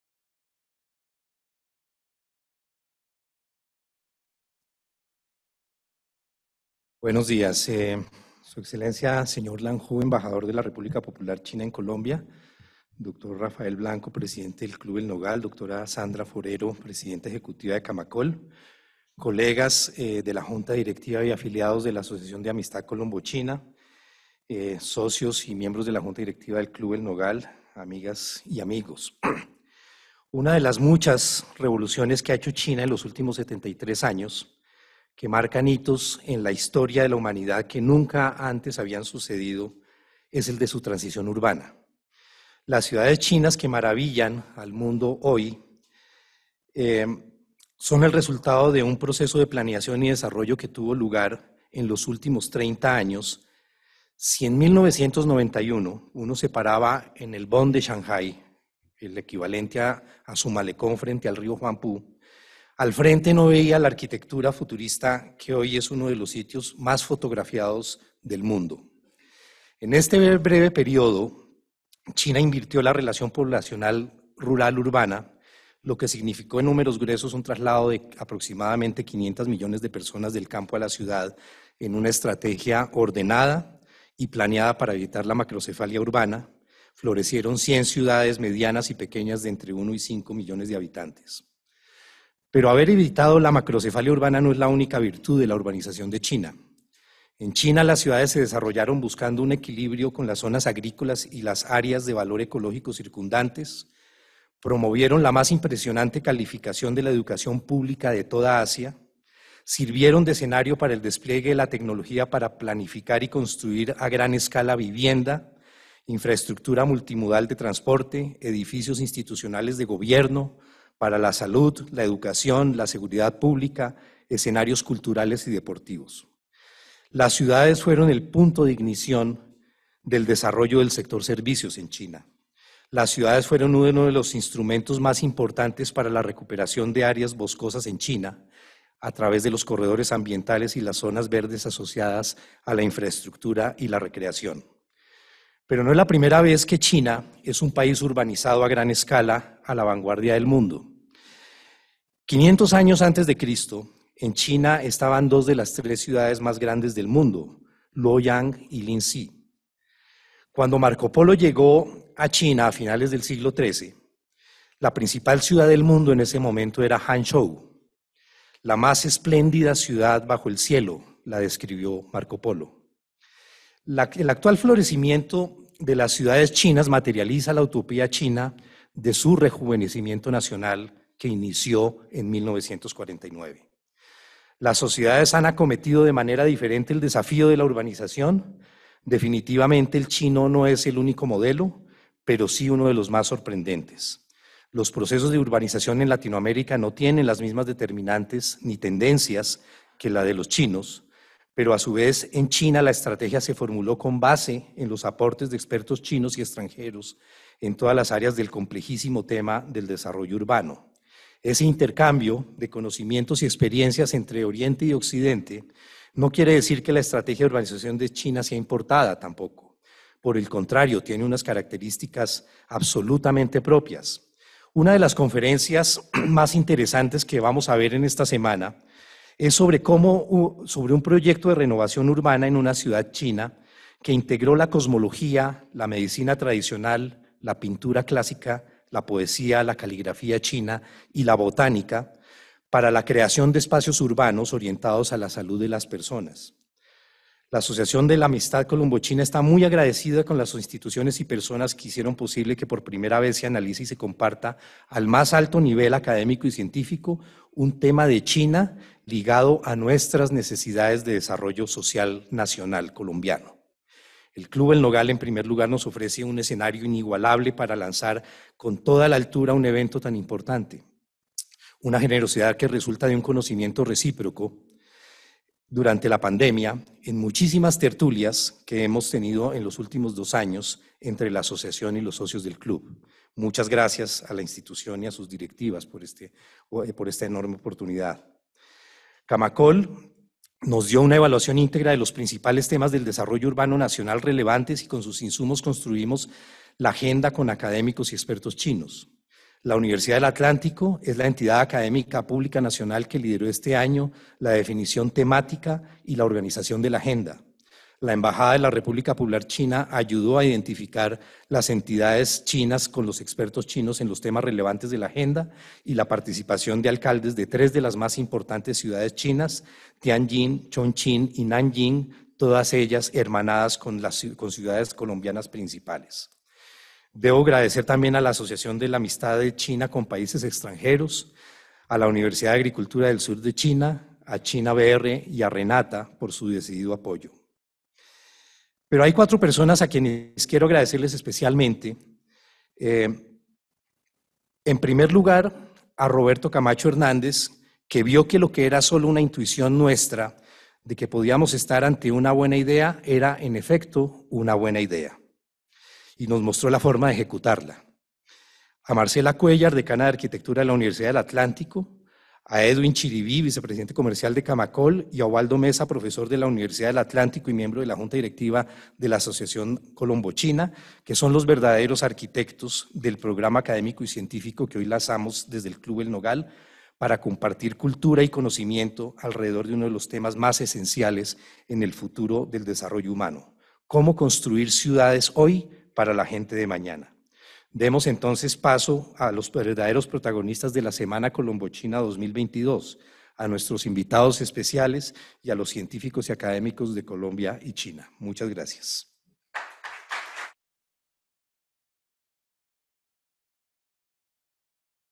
4 Guillermo Puyana Presidente Asociación de la Amistad Colombo China
Semana Colombo China 2022 Planeación y Desarrollo Urbano Ciudades de Hoy para la Gente del Mañana